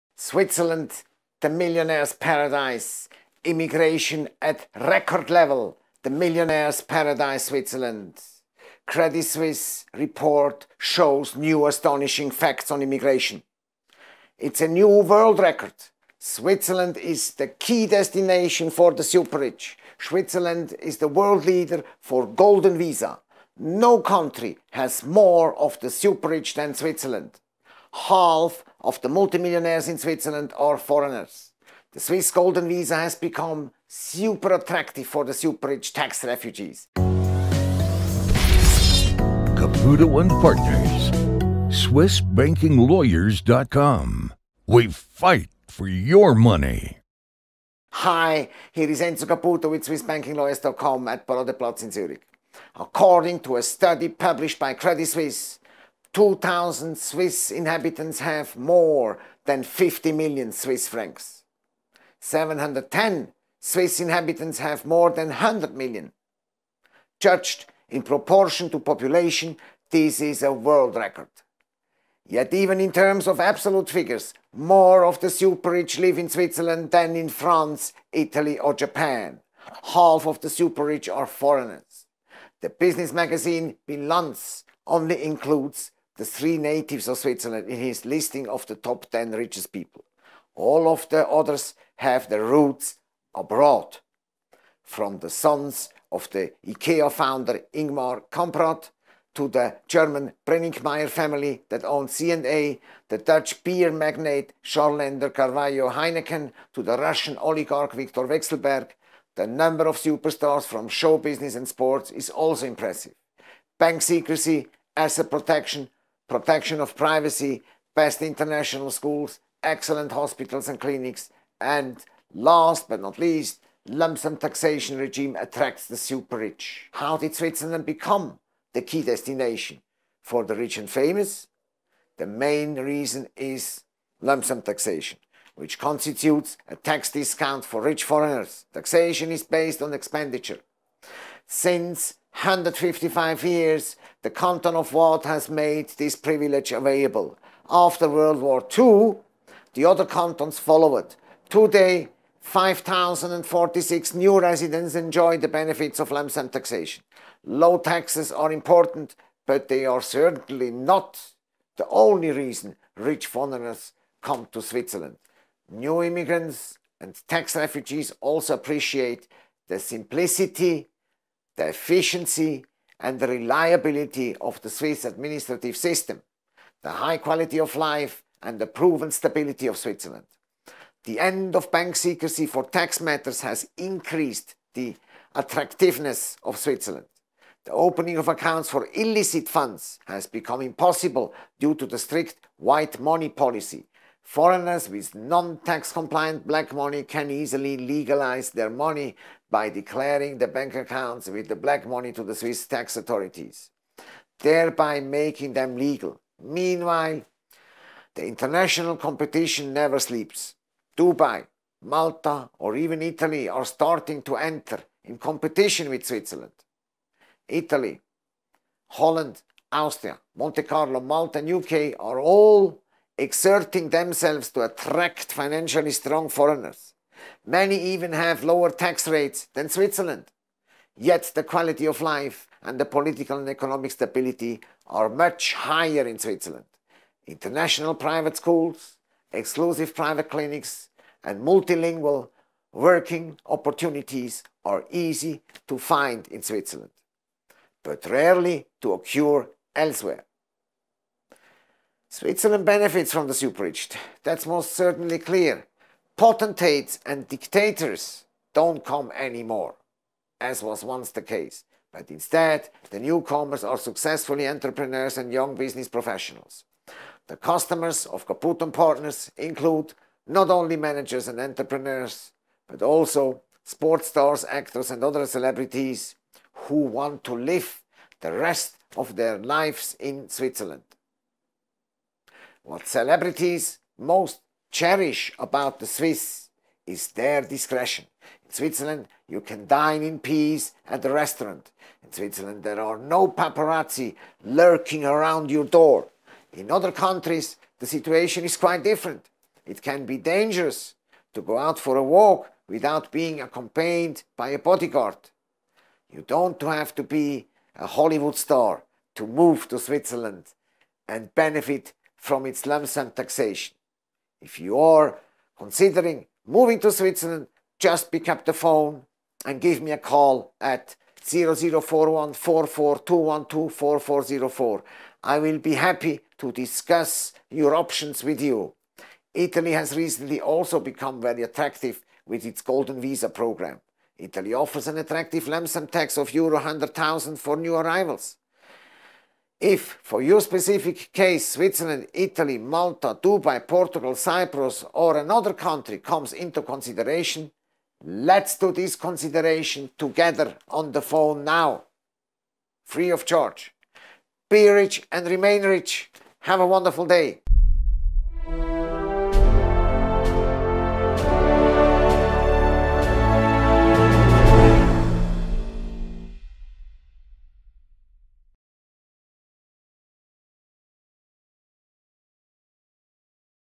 Super-rich are moving to Switzerland with Golden Visa (New Record!)
A Paradise for Millionaires: Immigration at Record Level

The billionaire’s paradise Switzerland – Credit Suisse report shows new facts on immigration.

New World Record: Switzerland is the key-destination for the super-rich, the world-leader for Golden Visa.

No country has more of the super-rich than Switzerland.
Half of the multimillionaires in Switzerland are foreigners.
Its Swiss Golden Visa provides a welcoming culture for the super-rich tax-refugees.

⇨ Multilingual service
⇨ 24/7 support